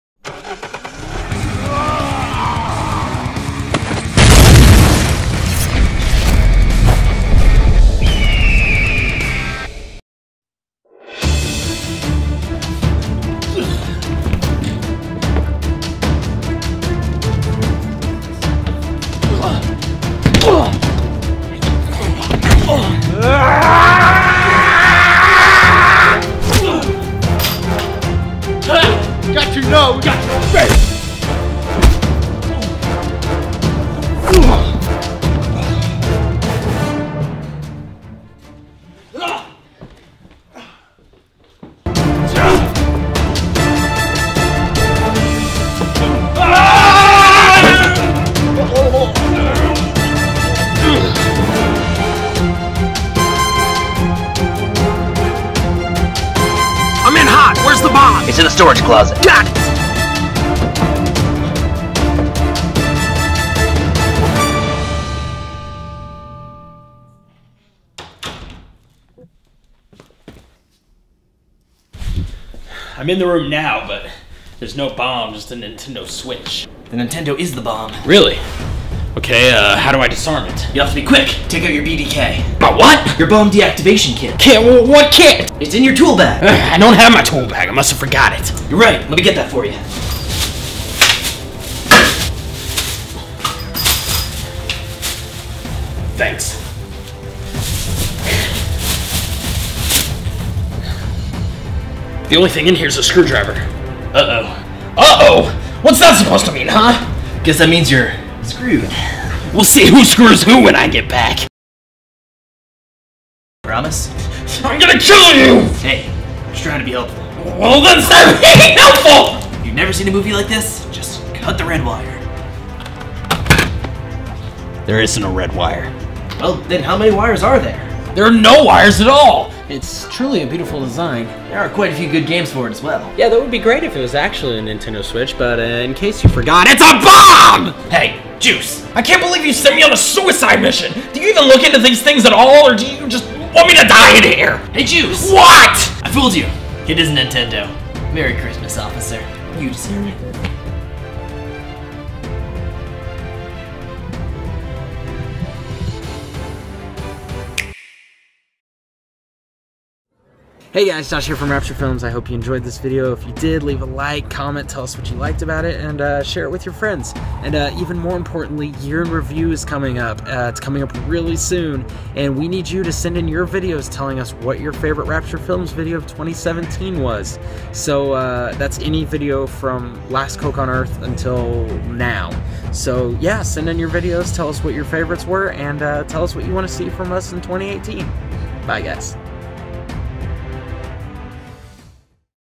Weirdest Commercial Ever
Action Juice returns to save the day yet again. This time, a bomb that could level a city block has been planted in a warehouse. However, upon arrival, Juice realizes that things aren't exactly as they seem. The bomb appears to be *gasp* a Nintendo Switch?! Will Juice ever disarm this bomb, or will he find himself spending countless hours in Odyssey first? Watch our latest Rapture Films video to find out!

Shot on a Canon Rebel T5i